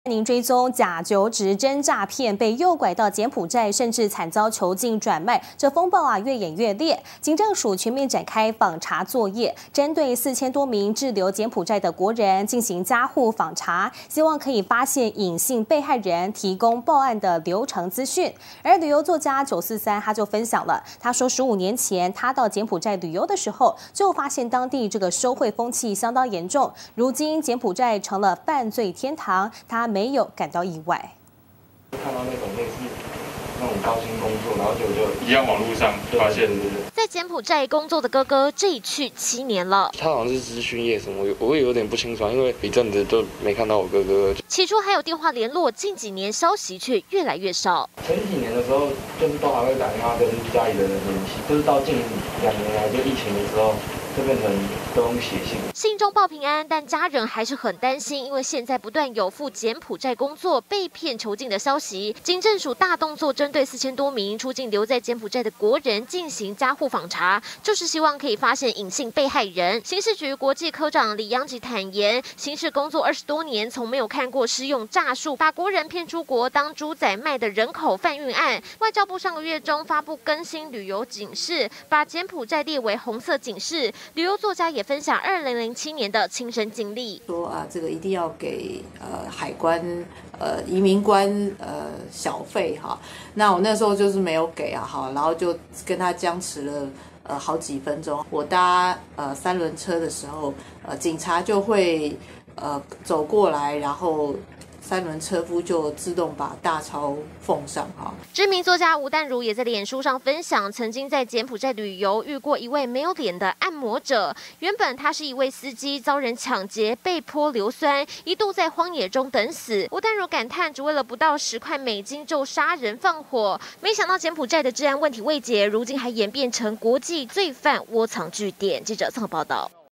假求職詐騙被拐到柬埔寨 警政署全面訪查
假求職詐騙，被誘拐到柬埔寨，甚至慘遭囚禁、轉賣，風暴越演越烈，警政署全面展開「訪查作業」，針對4千多名滯留柬埔寨的國人，進行家戶訪查，希望可以發現「隱性被害人」，提供報案流程資訊。而旅遊作家943就回憶，15年前，到柬埔寨旅遊，就發現當地「收賄風氣」嚴重，如今柬埔寨成了犯罪天堂，也不會感到意外。